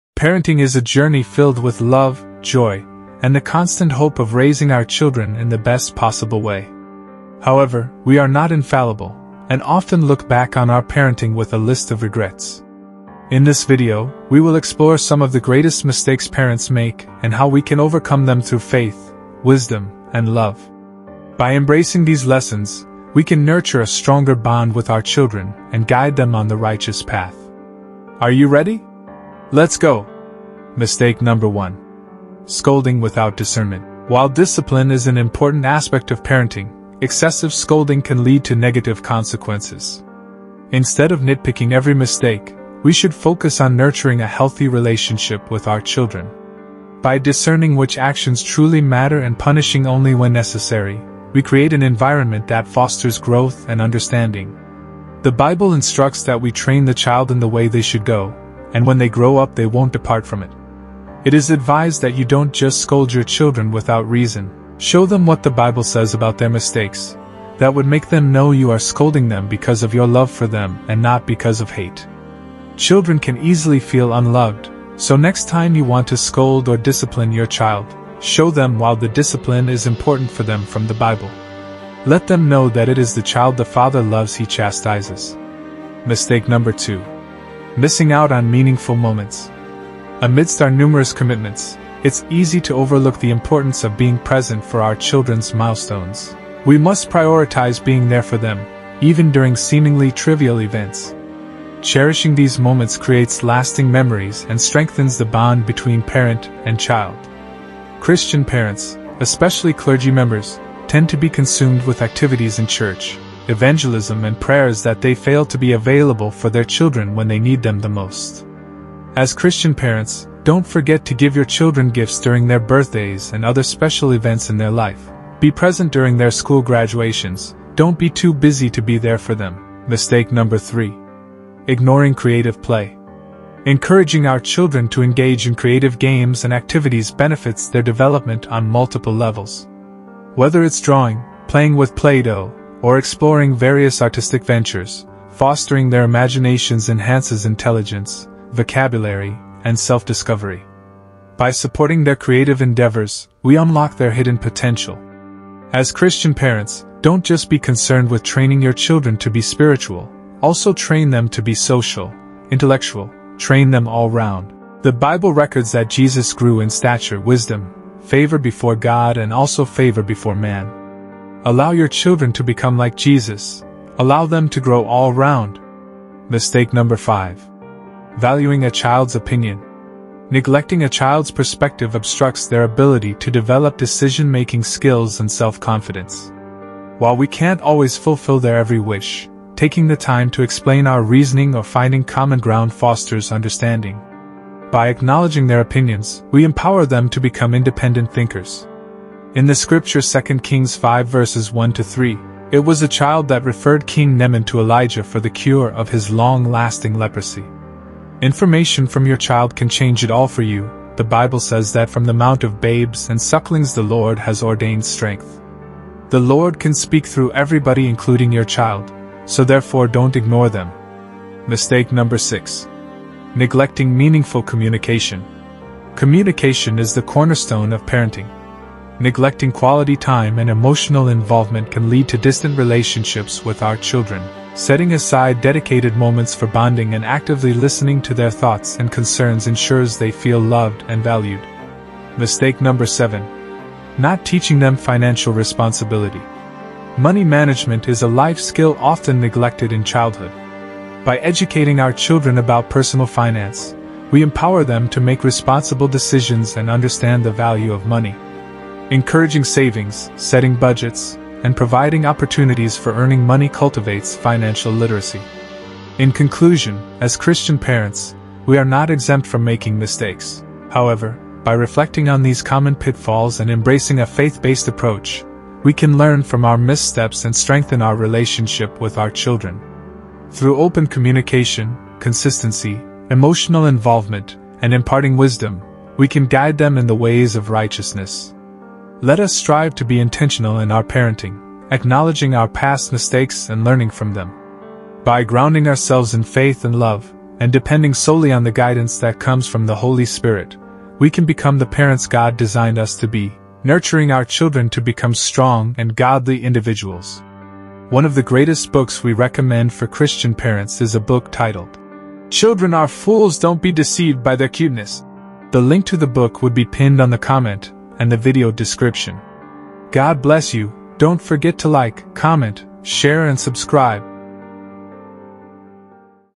How to Raise Godly Children: Christian Parenting Tips and Advice | Biblical Parenting
One of the best book for Christian Parenting

Raising Godly Children: Christian Parenting Tips and Advice | Biblical Parenting

Discover valuable insights and practical guidance in this enriching video on Christian parenting. Whether you're a new parent or have been on this journey for a while, join us as we delve into the depths of Christian parenting and explore how to raise children with biblical principles.

Drawing from the wisdom and teachings of Christianity, we provide a wealth of tips and advice to help you navigate the challenges and joys of parenting. From establishing a foundation of faith to communicating effectively with your children, we cover a wide range of topics to support and empower you in your role as a Christian parent.

As a homeschool mom, our host offers first-hand knowledge and experiences, sharing valuable tips specific to the homeschooling environment. Learn how to create a nurturing and spiritually grounded atmosphere within your home, fostering a strong Christian foundation for your children's growth and development.

Embrace the transformative power of biblical parenting as we explore key principles and practices rooted in Christian teachings. Discover how to instill virtues, discipline with love and grace, and cultivate a deep connection with your children based on trust and mutual respect.

Whether you're seeking guidance on discipline strategies, maintaining a healthy balance between nurturing and authority, or incorporating faith into everyday life, this video aims to provide practical solutions and inspiration for your Christian parenting journey.